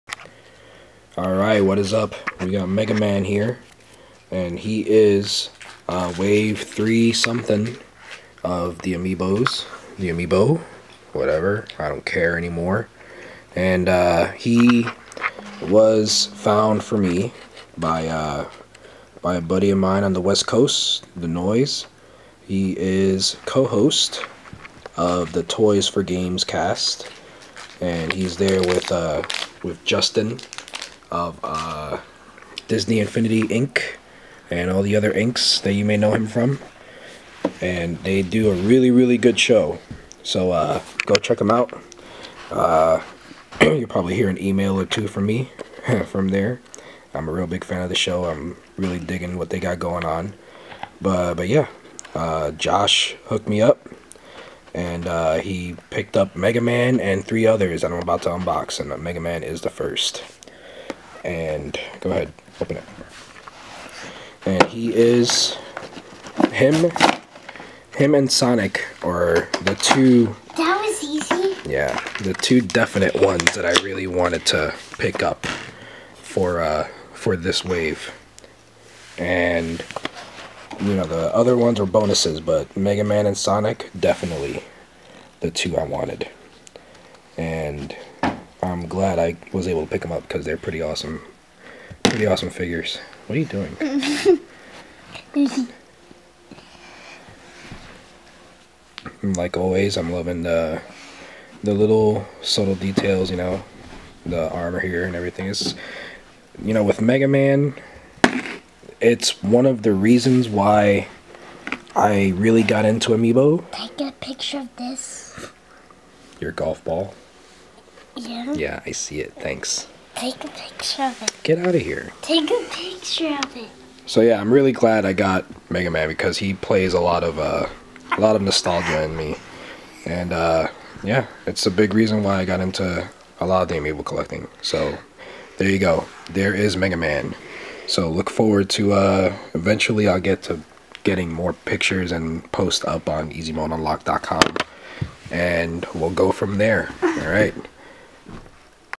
Mega Man amiibo Unboxing!
Thanks to @thenoyse from the Toys 4 Games Cast, I got a hold of 4 new amiibo! First up, Mega Man!

Out of this current wave, Mega Man is one of the 2 I knew I definitely wanted to pick up. I have too many memories with the blue bomber to not get him.